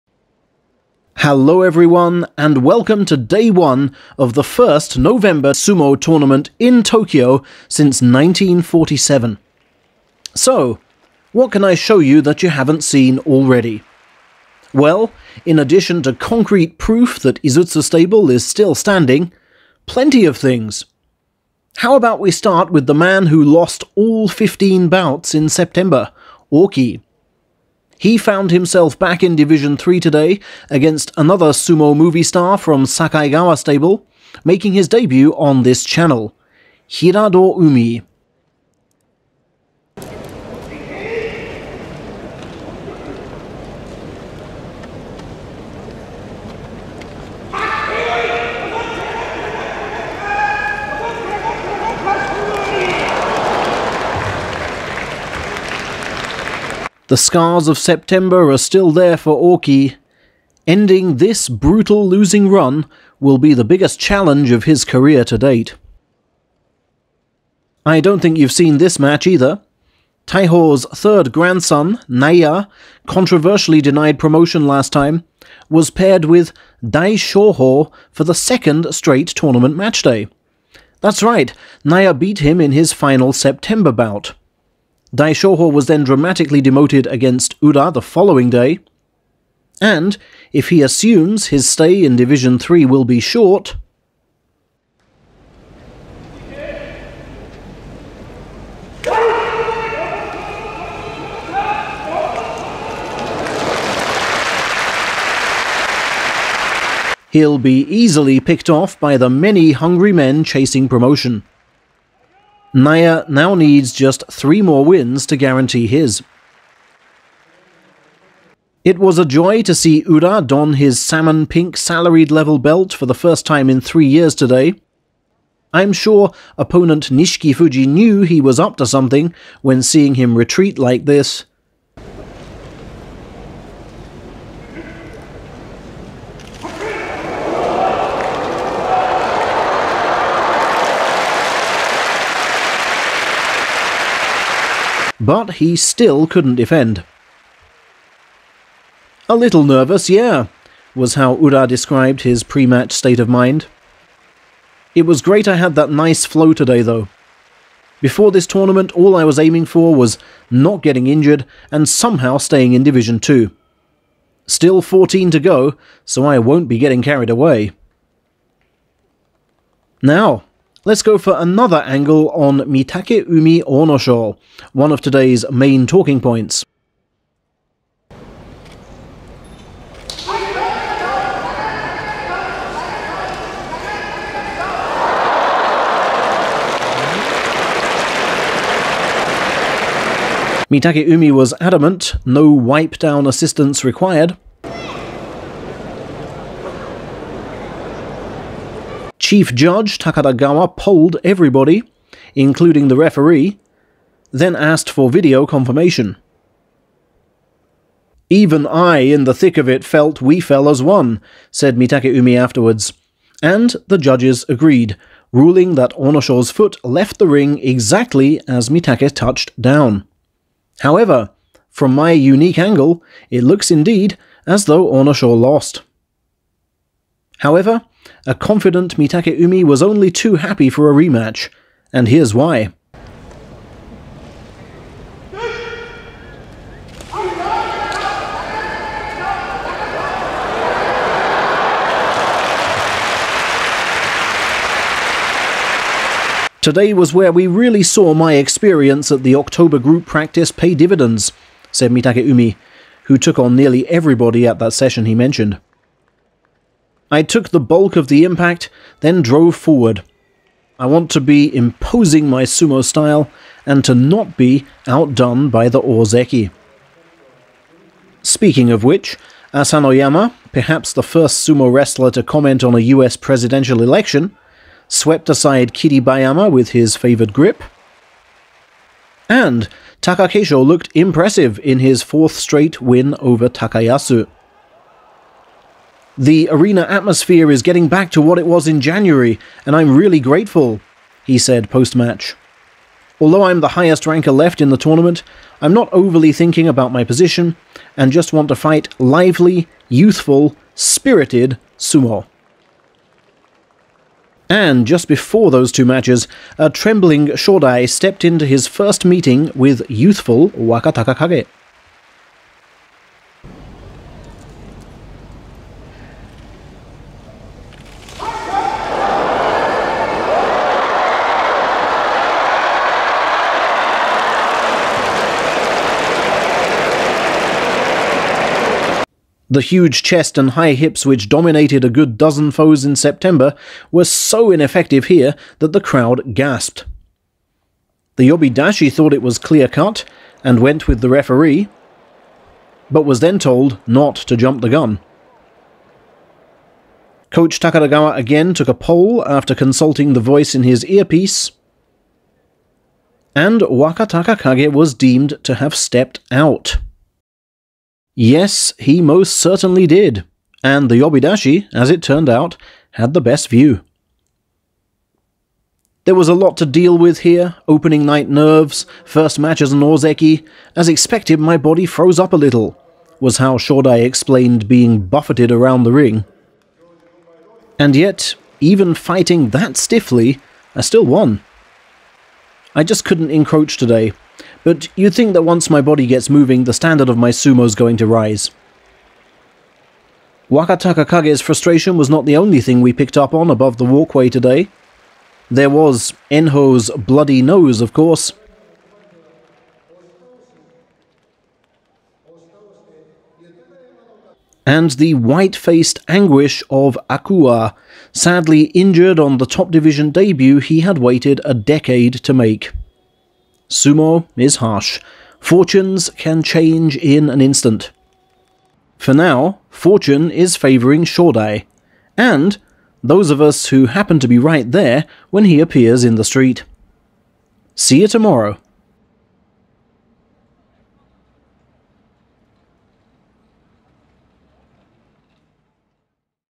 Sumo Day 1: The bouts and angles you didn't see!
You've likely seen a lot of sumo today - but certainly not everything! I bring you bouts you haven't seen, alternative camera angles of close calls (one of which the judges got wrong) and all the other stuff you can only pick up if you're actually there.